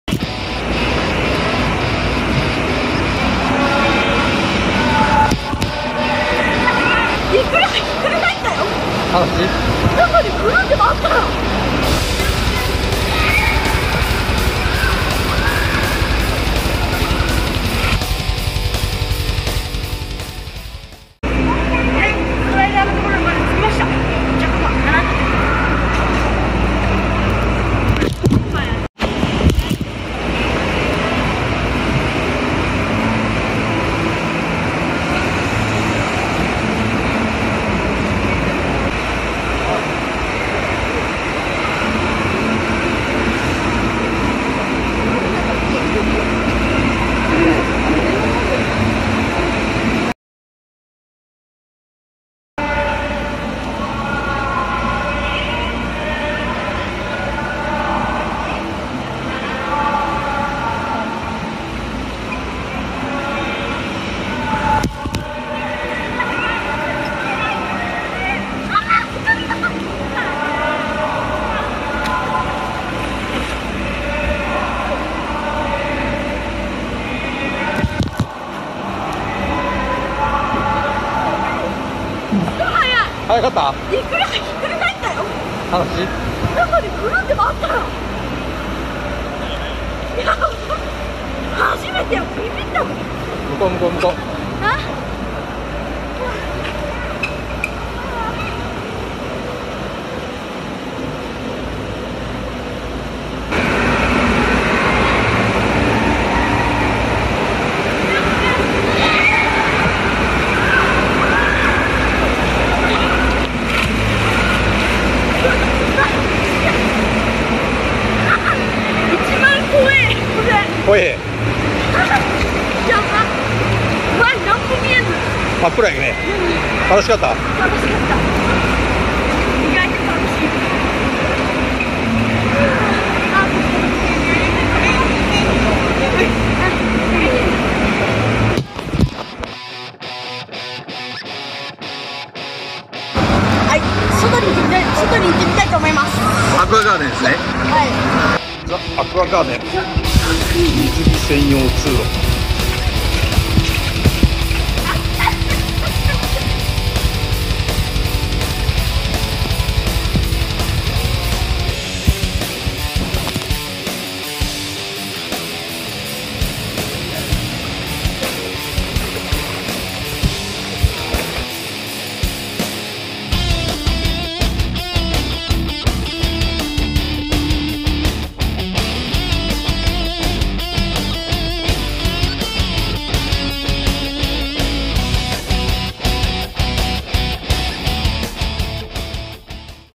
【アクアビート・スライダー😝】
今回はアクアビートのスライダーを滑ります🤗
滑ってる時は撮影NGだそぅです⤵
100m位あるんで楽しかった🎵